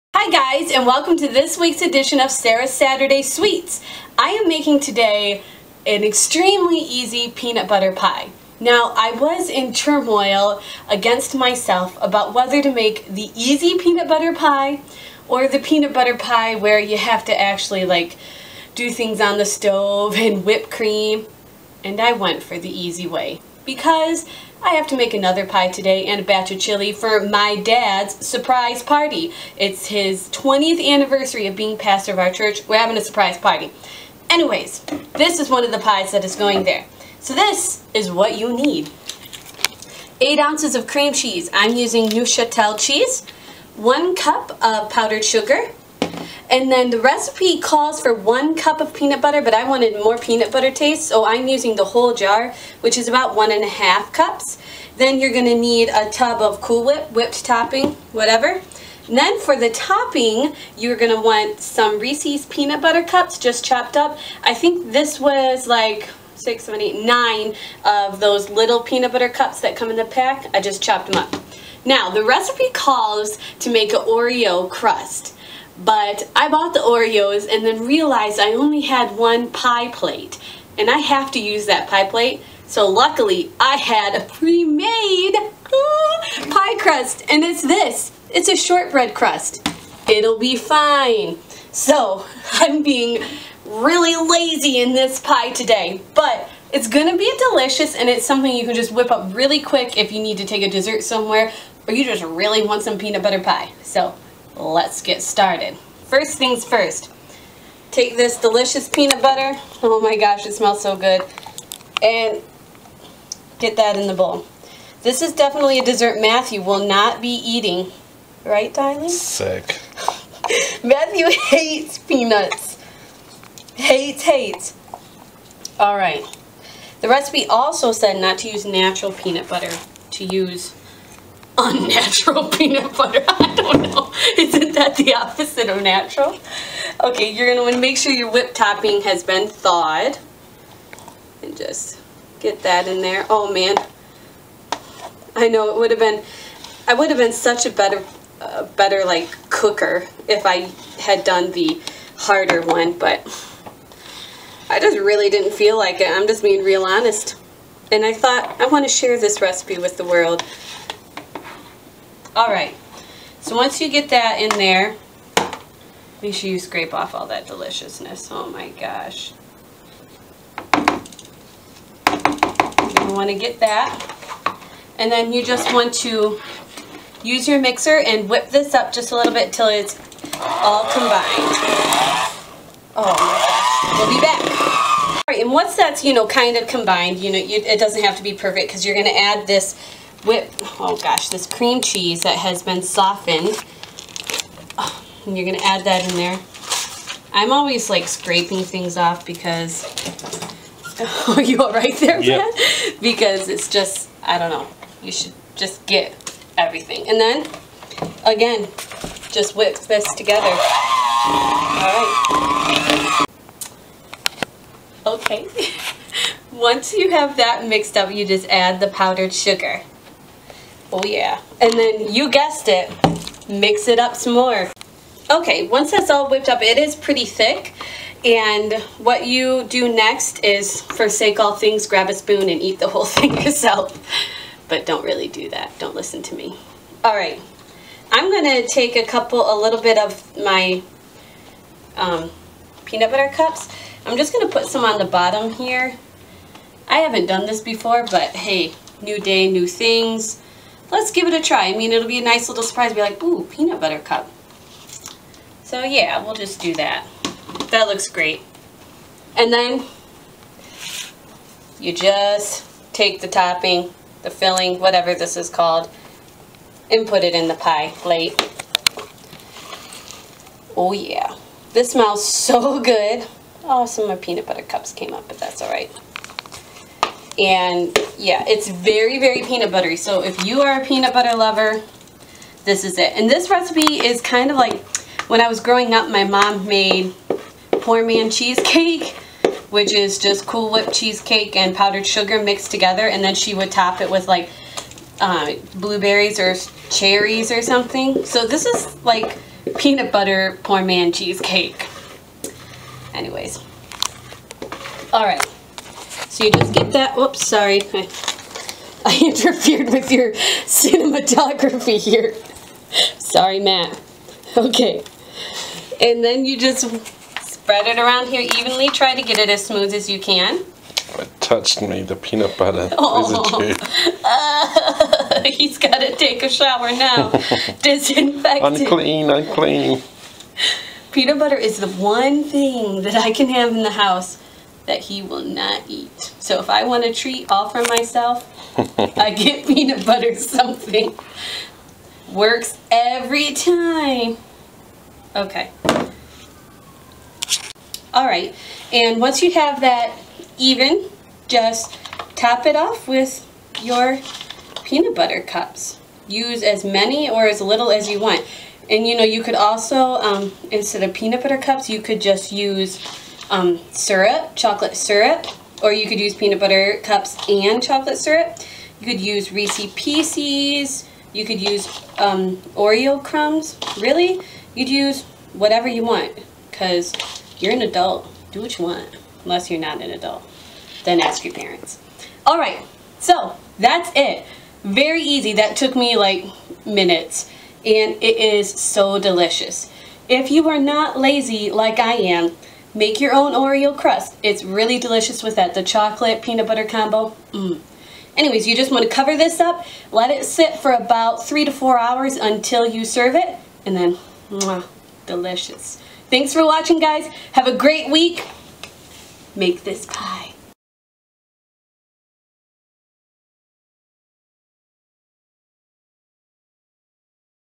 Sara's Saturday Sweets - Easy Peanut Butter Pie
This is a really easy Peanut Butter Pie that probably has a bagillion calories in it! Sorry I didn't figure them out! Give it a try and share it with some friends.

Here is the recipe I kinda sorta used -